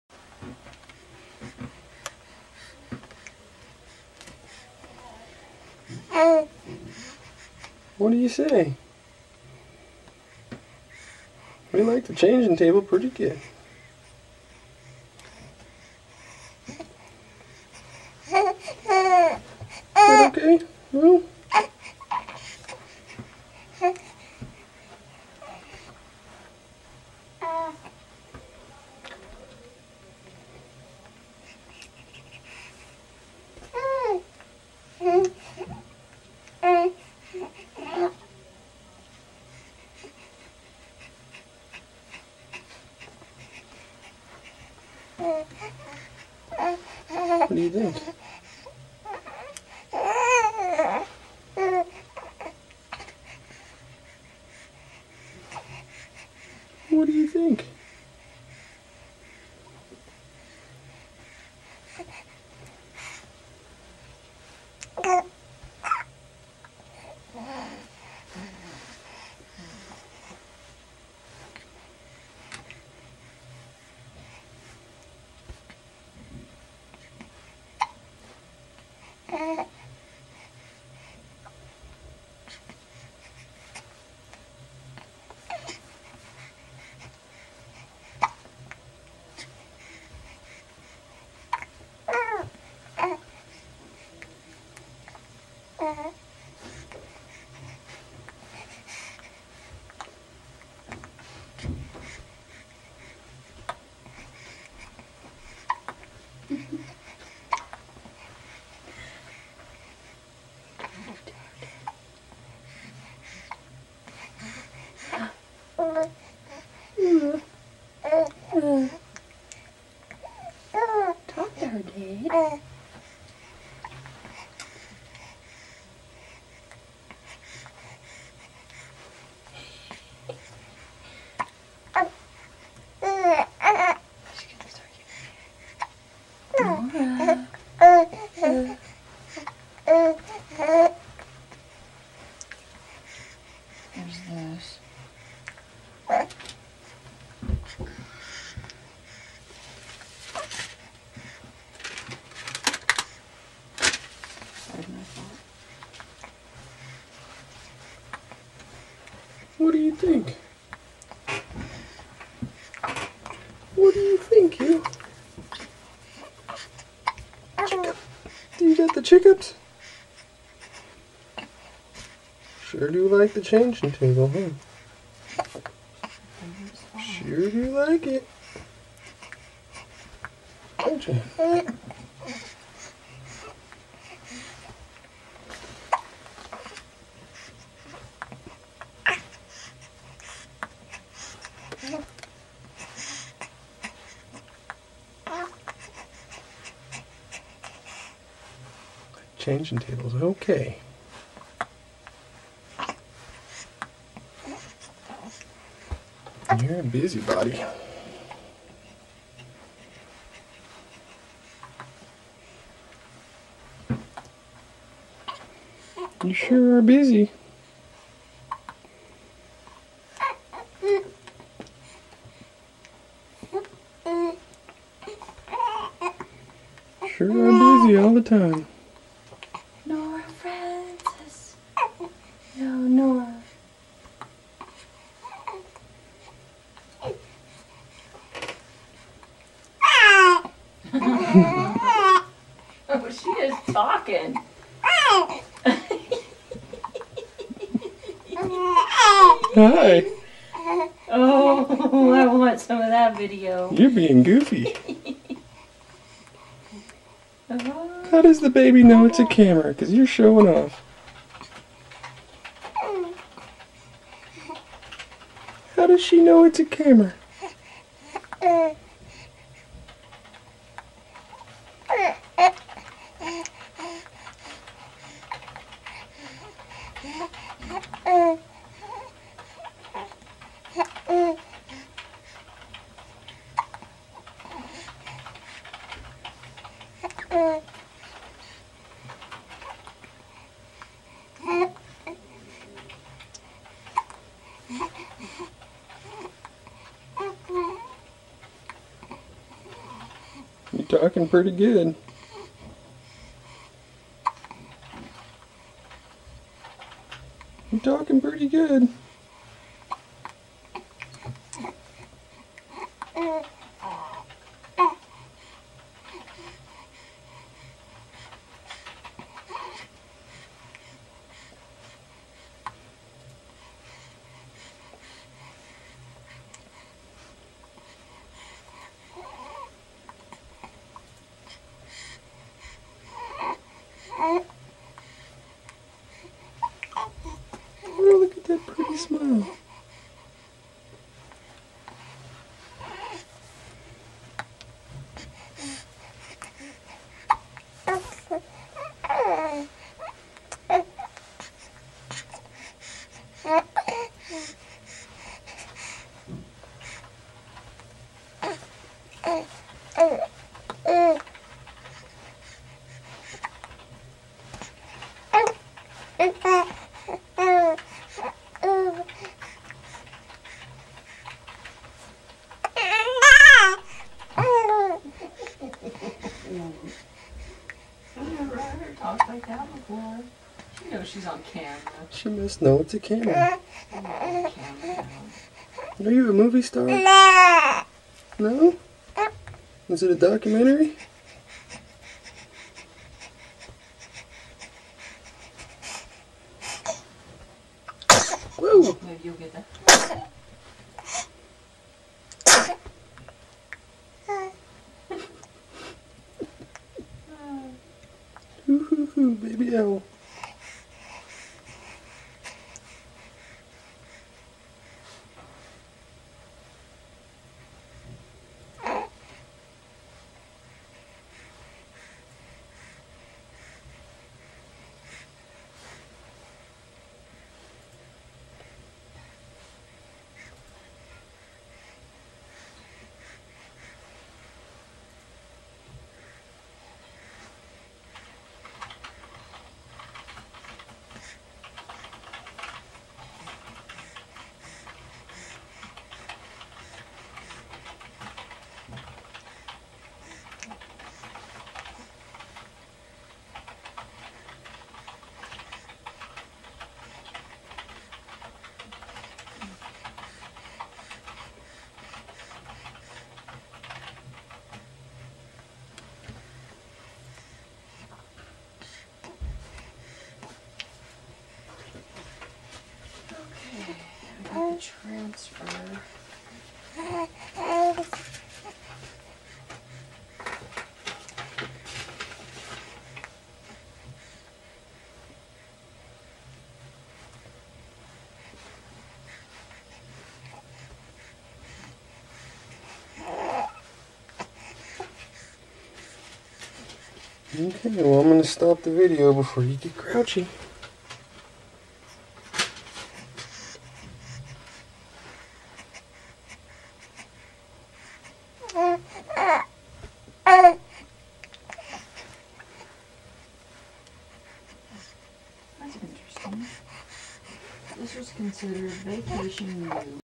nora frances white
this is our daughter having a bit of fun on the 'ol changing table (her favorite place for some reason)